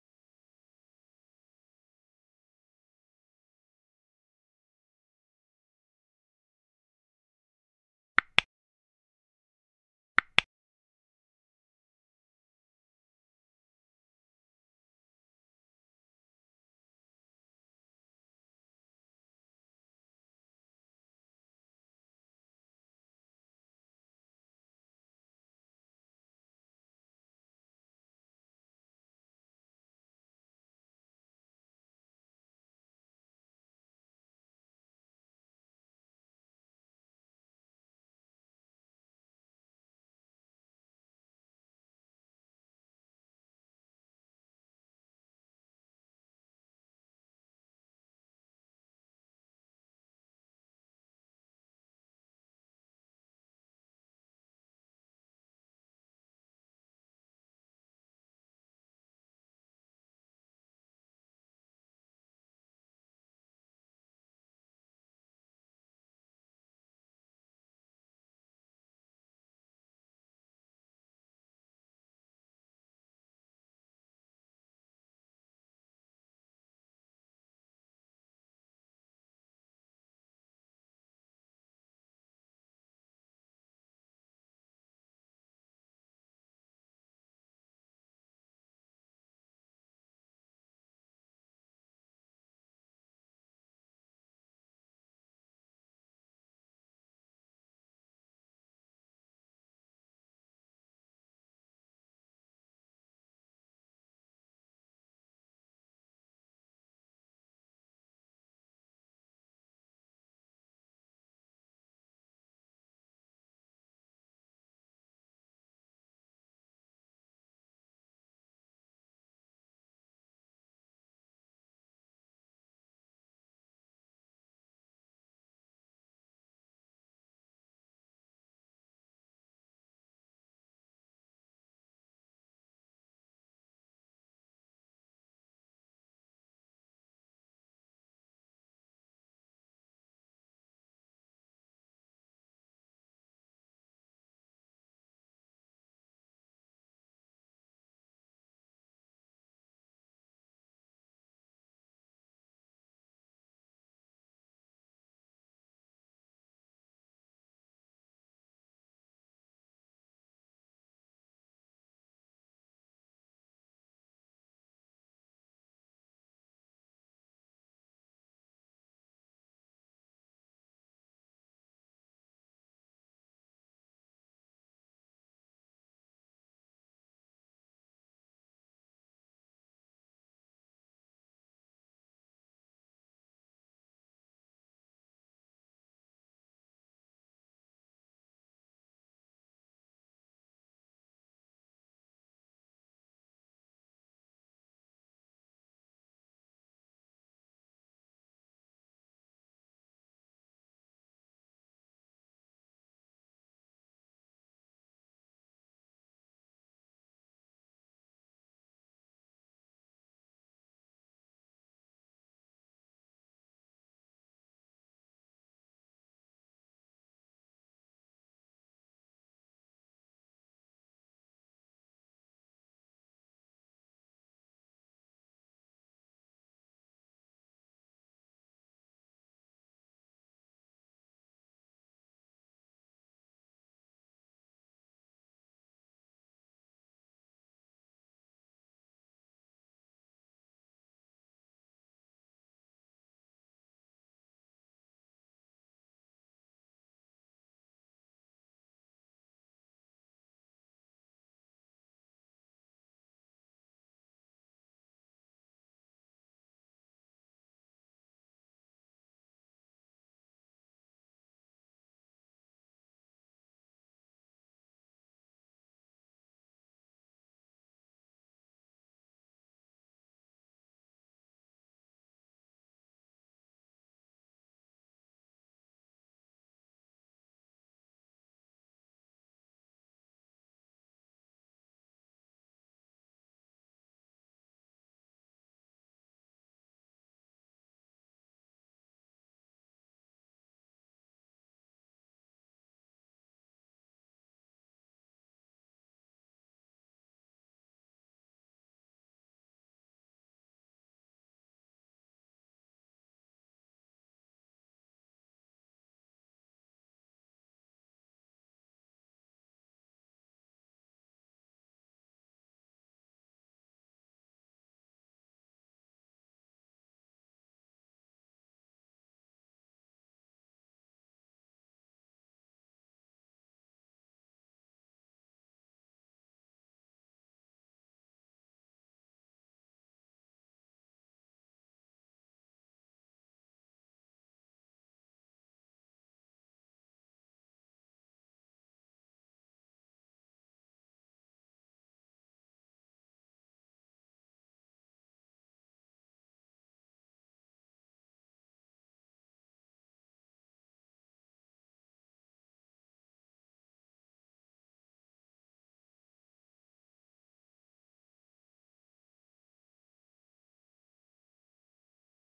3EME PARTIE DE QCM de la Loi Cadre 06-22, Préparation aux EAP , concours de recrutement.
QCM de la Loi Cadre 06-22, Préparation aux EAP , concours de recrutement des professionnels de santé